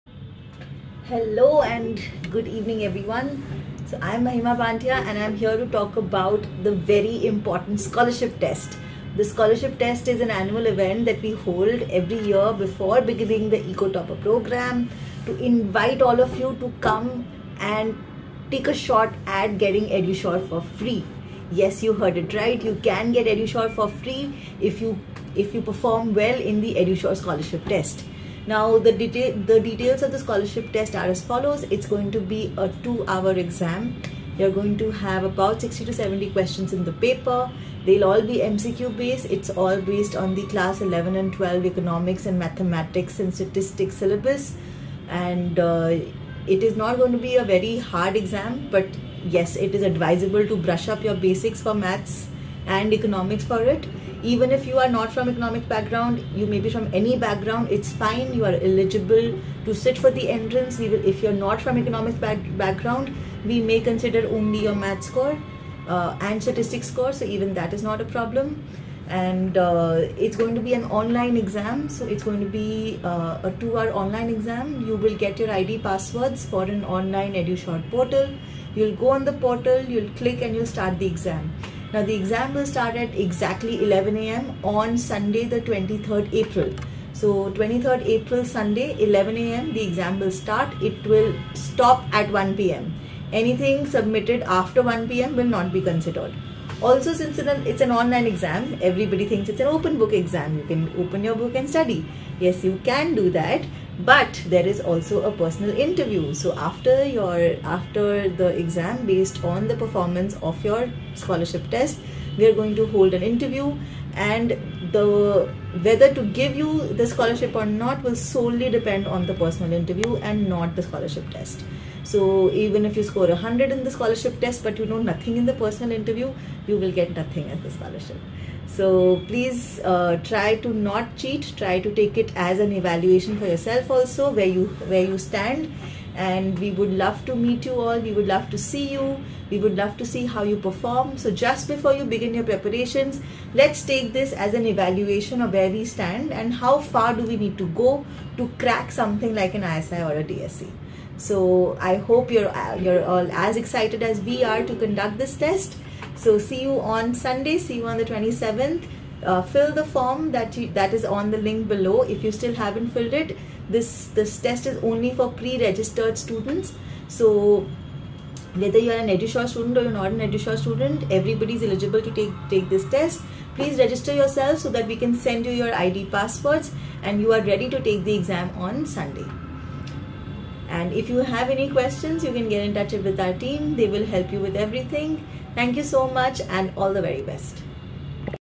EduSure Scholarship Test 2023| Chance to get EduSure Courses for free
All details related to the Scholarship test- Golden Opportunity!!

It is an annual event organized by *Samkith Sir & Mahima Ma'am* before launching the *Eco Topper Programme'24*.

You can get EduSure for *FREE*‼️

_*How*_?

Perform well in the EduSure *Scholarship Test* and the *Interview* that follows.📝

Registration Mandatory!

For further assistance please contact EduSure team on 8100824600/01.